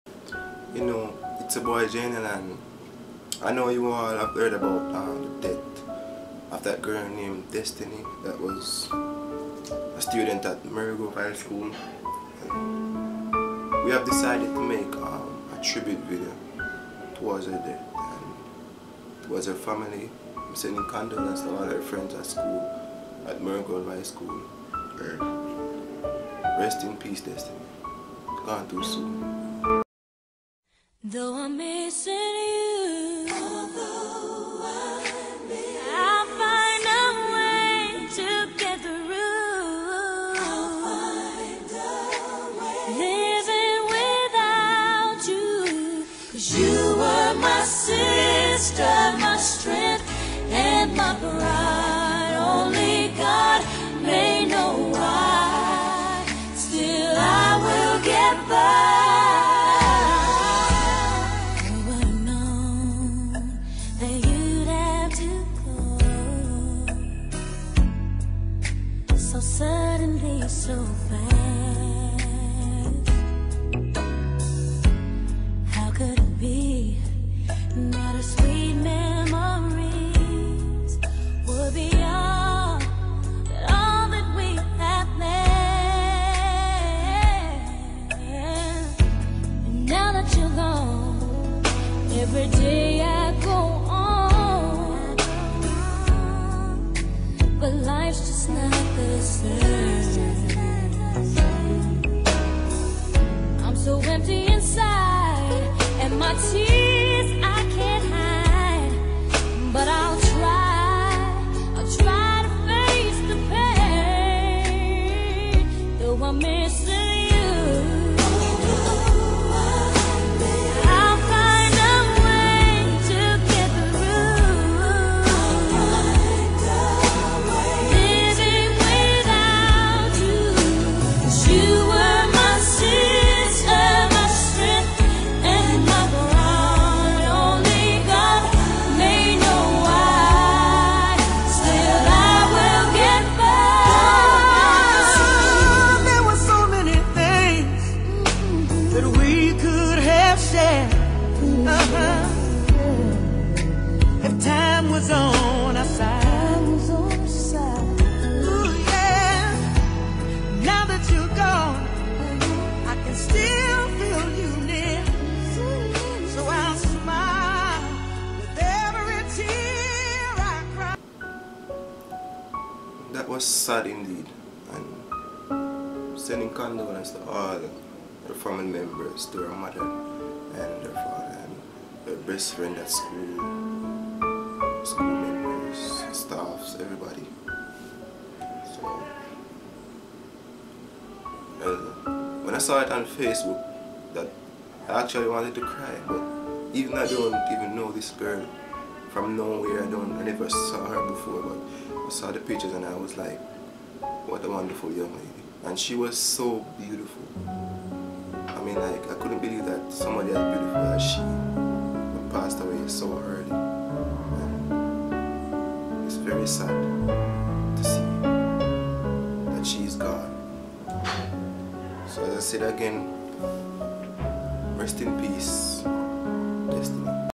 R.I.P DESTINY..jnel tv tribute video
Destiny was an attendant at the Merlgrove high school who has passed away a few days now she have been missed by friends and family mother and father and even by me help spread this sad story across the world simply by like sharing this video and feel free to subscribe jnel tv thank you. R.I.P DESTINY !!! :(